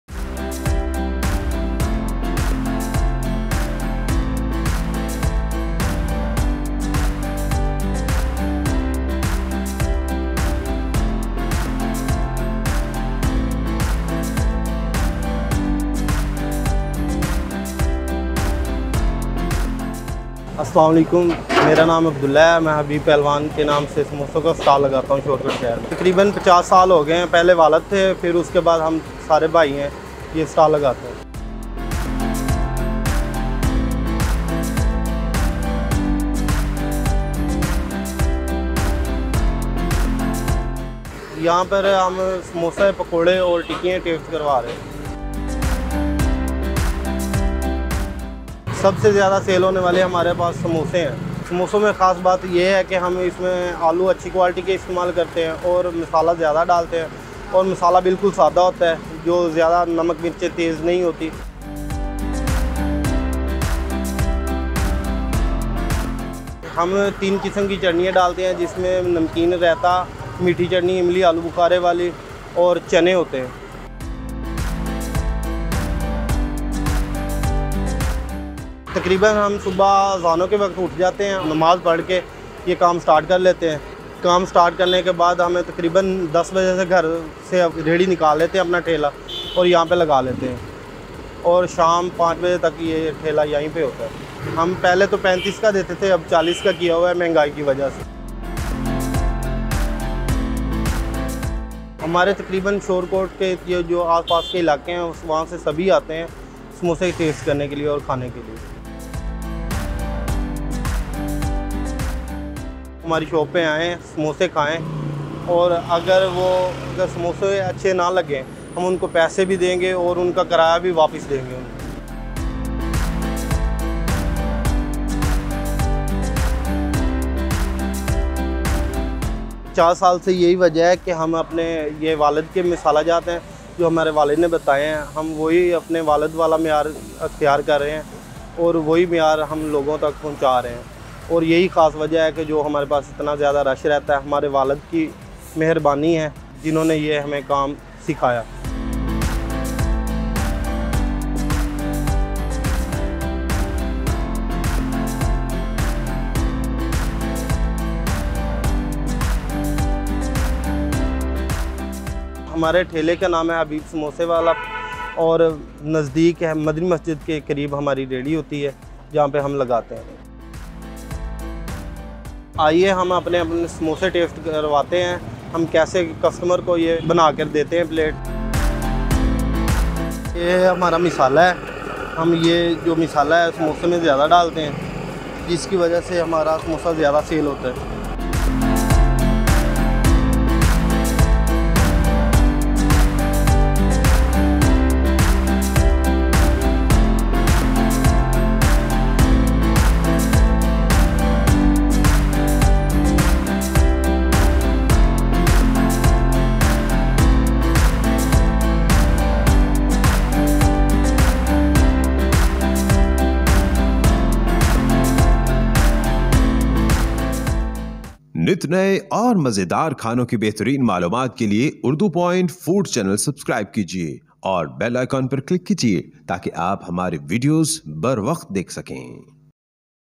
50 years old Shop Of A Pehlwan - Famous Potatoes Samosas In Shorkot
Habib Pehalwan Samosay wala is located in Shorkot. They have been selling somosa, pakoray and tikki for the past 50 years. The small food cart is run by the three brothers, who have tried to maintain their taste in the long run. Their samosa are famous in the whole city and the sauces and chutney are the main enhancers of the taste. How they make samosa and which ingredients they use, let us know in this video.
Report: Usman Ali